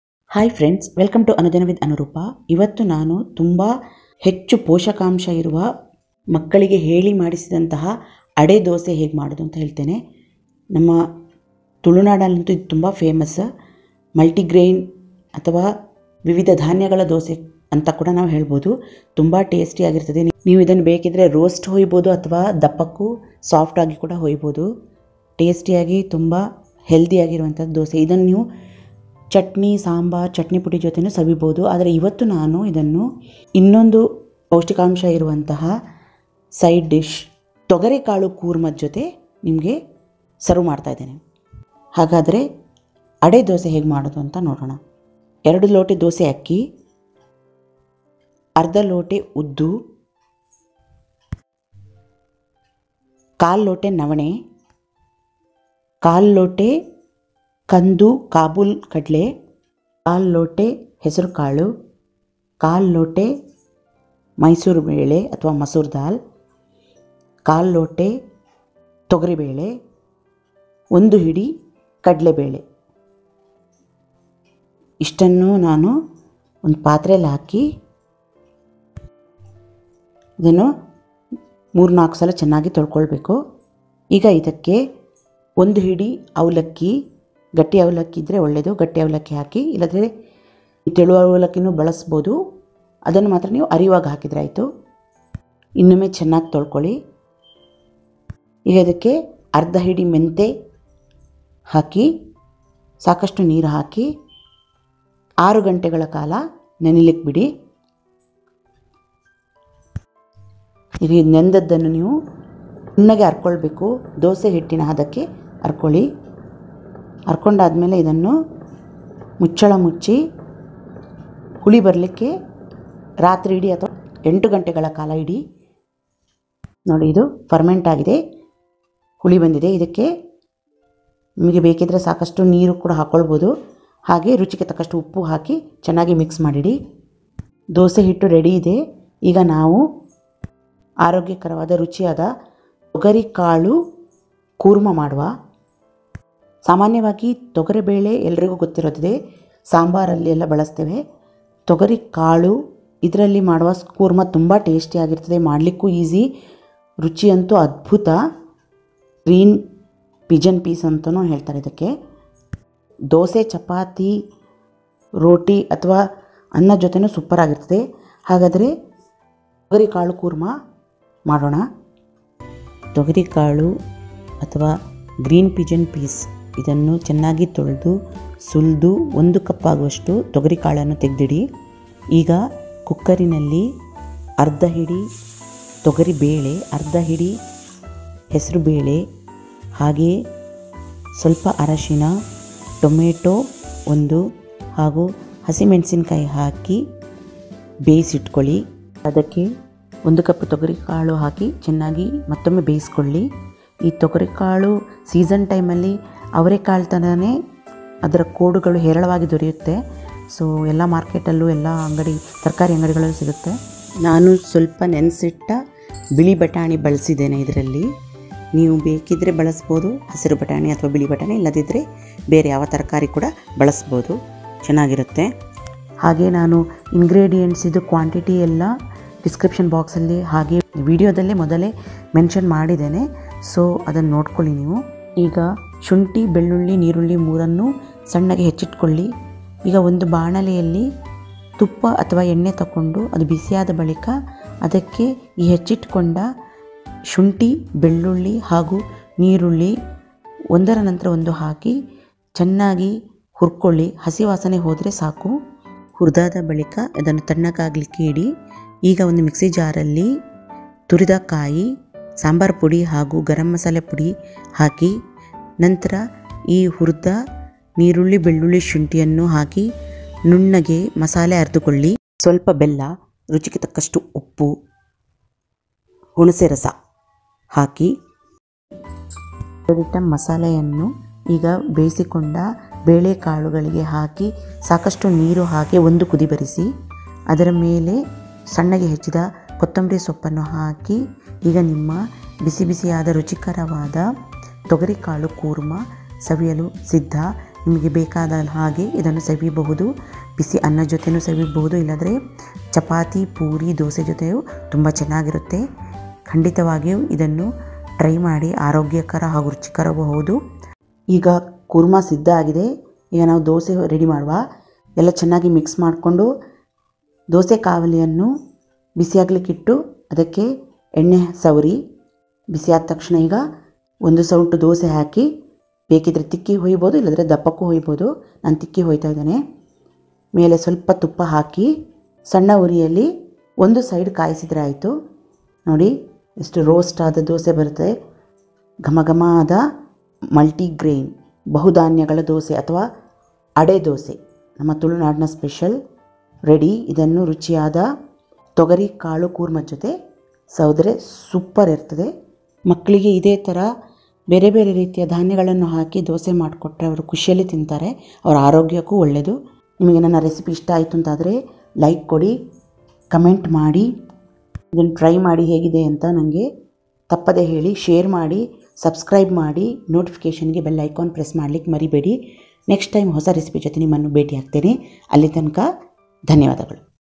ADE DOSA/MULTI GRAIN DOSA/ಅಡೆ ದೋಸೆ With GREEN PIGEON PEAS KOORMA/ತೊಗರಿಕಾಳು ಕೂರ್ಮ/तुअर दाल कोरमा
(Recipe both in English and Kannada)

Hi friends.

Ade_Dosa or Multigrain_Dosa is very tasty healthy as well as easy to make. Kids favourite. It's very high protein dosa so from this you can easily make kids to enjoy multi grains and be healthy, strong too. You can enjoy this dosa with Chutney Sambar or chutney powder. Today I am serving Ade Dosa with GREEN_PIGEON_PEAS_KOORMA which is once again healthy combo 😋👌👍.

ಅಡೆ_ದೋಸೆ ಅಥವಾ ಬಹುಧಾನ್ಯಗಳ ದೋಸೆ ತುಂಬಾ ರುಚಿ, ಆರೋಗ್ಯಕರ ಮತ್ತು ಮಕ್ಕಳ ನೆಚ್ಚಿನದ್ದು ಹಾಗೇ ಮಾಡಲೂ ಸುಲಭ. ಇದು ತುಂಬಾ ಹೆಚ್ಚಿನ ಪೋಷಕಾಂಶ ಇರುವ ದೋಸೆಯಾಗಿದ್ದು ಸುಲಭವಾಗಿ ಮಕ್ಕಳನ್ನು ಬಹು ಧಾನ್ಯಗಳನ್ನು ಆನಂದಿಸುವಂತೆ ಮಾಡಬಹುದು ಹಾಗೇ ಆರೋಗ್ಯದ ಬೆಳವಣಿಗೆಗೂ ಉತ್ತಮ. ಮತ್ತು ನೀವು ಈ ದೋಸೆಯನ್ನು ಚಟ್ನಿ, ಸಾಂಬಾರ್ ಅಥವಾ ಚಟ್ನಿ ಪುಡಿಯೊಂದಿಗೆ ಆನಂದಿಸಬಹುದು. ಇಂದು ನಾನು ಅಡೆ ದೋಸೆಯನ್ನು ತೊಗರಿ_ಕಾಳು_ಕೂರ್ಮ ಜೊತೆಗೆ ಬಡಿಸುತ್ತಿದ್ದೇನೆ. ಇದು ಮತ್ತೊಮ್ಮೆ ಆರೋಗ್ಯಕರವಾದ ಕಾಂಬೊ 😋👌👍.

      Toor Dal is an integral part of cooking usually in preparation of Sambar or Curry. Today I have an unique recipe which is made from Toor Dal Seed/Green Pigeon Peas. So let's see spicy yummy delicious Green Pigeon Peas Koorma which is a perfect side dish with Chapathi/Poori/Hot Rice.

     ತೊಗರಿ ಬೇಳೆ ಸಾಮಾನ್ಯವಾಗಿ ಸಾಂಬಾರ್, ಸಾರು ತಯಾರಿಕೆಯಲ್ಲಿ ಬಳಸುವುದು. ಇಂದು ನಾನು ಒಂದು ವಿಶಿಷ್ಟವಾದ ರುಚಿ ತಂದಿದ್ದೇನೆ. ತೊಗರಿ ಕಾಳಿನಿಂದ ತಯಾರಿಸಲಾಗುವ ರುಚಿಕರವಾದ ಹೆಸರು ಕಾಳಿನ ಕೂರ್ಮಾವನ್ನು ಹೇಗೆ ಮಾಡೋದಂತ ನೋಡೋಣ. ಇದು ಚಪಾತಿ / ಪೂರಿ / ಬಿಸಿ ಬಿಸಿ ಅನ್ನದೊಂದಿಗೆ ಸೂಪರ್.

Ade_Dosa/ಅಡೆ_ದೋಸೆ
Ingredients/ಸಾಮಾಗ್ರಿಗಳು :
Dosa Rice/ದೋಸೆ ಅಕ್ಕಿ   2 Glasses/ಲೋಟಗಳು
Urad/ಉದ್ದು                            1/2 Glass/ಲೋಟ
Indian Millet/ನವಣೆ               1/4 Glass/ಲೋಟ
Brown Channa/ಕಂದು ಕಾಬೂಲ್ ಕಡಲೆ  1/4 Glass/ಲೋಟ
Green Gram/ಹೆಸರು ಕಾಳು     1/4 Glass/ಲೋಟ
Masoor Dal/ಮೈಸೂರು ಬೇಳೆ  1/4 Glass/ಲೋಟ
Toor Dal/ತೊಗರಿ ಬೇಳೆ            1/4 Glass/ಲೋಟ
Channa Dal/ಕಡಲೆ ಬೇಳೆ             1 Handful/ಹಿಡಿ
Flatten Rice/ಗಟ್ಟಿ ಅವಲಕ್ಕಿ          1 Handful/ಹಿಡಿ
Fenugreek Seeds/ಮೆಂತೆ       1/2 Handful/ಹಿಡಿ
Salt per taste/ರುಚಿಗೆ ಉಪ್ಪು
Water/ನೀರು

Ingredients/ಸಾಮಾಗ್ರಿಗಳು :
Toor dal + Moong dal/ತೊಗರಿ ಬೇಳೆ + ಹೆಸರು ಬೇಳೆ                              one handful/ಒಂದು ಹಿಡಿ
Tomato/ಟೊಮೇಟೊ                                          1
Green chilly/ಹಸಿ ಮೆಣಸು                                 1
Turmeric Powder/ಅರಶಿನ ಪುಡಿ    1 tsp/ಚಮಚ
Green Pigeon Peas/ತೊಗರಿ ಕಾಳು    1 cup/ಕಪ್
Ginger/ಶುಂಠಿ              Small piece/ಸಣ್ಣ ತುಂಡು
Onion/ಈರುಳ್ಳಿ                                                    1
Garlic cloves/ಬೆಳ್ಳುಳ್ಳಿ ಬೀಜಗಳು                    8-10
Oil/Ghee                                  to fry/ಹುರಿಯಲು
grated Coconut/ತುರಿದ ತೆಂಗಿನಕಾಯಿ   half hand full/ಅರ್ಧ ಹಿಡಿ
Sambar powder/ಸಾಂಬಾರ್ ಪುಡಿ   1 tsp/ಚಮಚ
Garam masala/ಗರಂ ಮಸಾಲೆ        1 tsp/ಚಮಚ
Tamarind juice/ಹುಣಸೆರಸ            1 tsp/ಚಮಚ
Little Jaggery/ಸ್ವಲ್ಪ ಬೆಲ್ಲ
Coriander leaves little/ಕೊತ್ತಂಬರಿ ಸೊಪ್ಪು ಸ್ವಲ್ಪ
Little Green/White Peas (optional)
ಬೇಕಾದ್ರೆ ಸ್ವಲ್ಪ ಬಟಾಣಿ
Water/ನೀರು